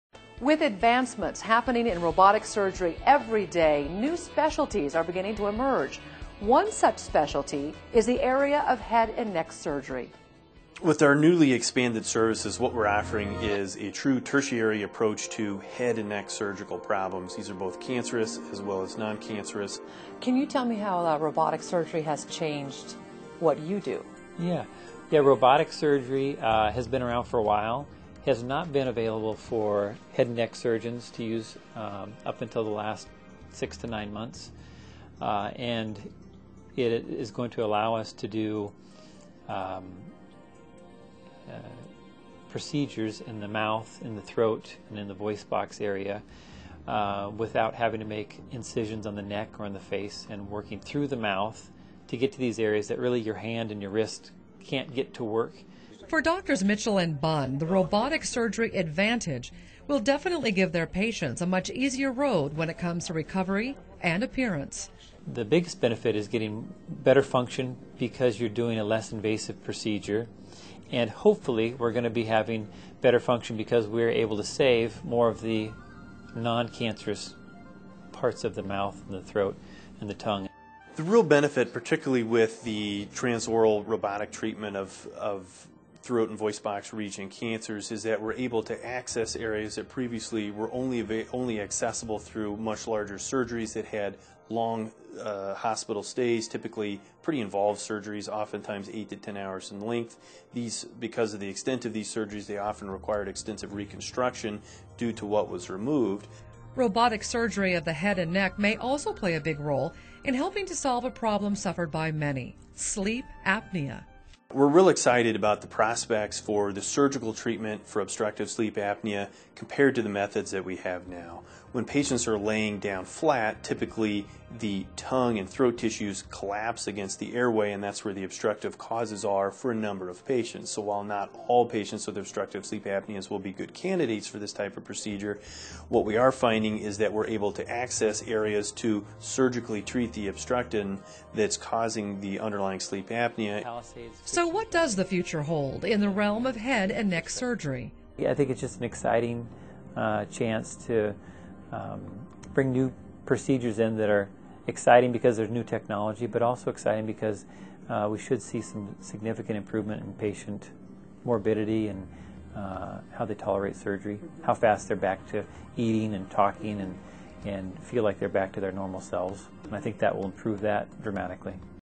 Robotic Head & Neck Surgery
With advancements happening in robotic surgery every day, new specialties are are beginning to emerge. Once such specialty is the area of head and neck surgery.

Brian Mitchell, DO and Jeff Bunn, MD, of Spokane ENT (ear, nose and throat), say robotic surgery allows them to access "from the inside out" to treat cancers of the mouth, throat and larynx, and well as obstructive sleep apnea.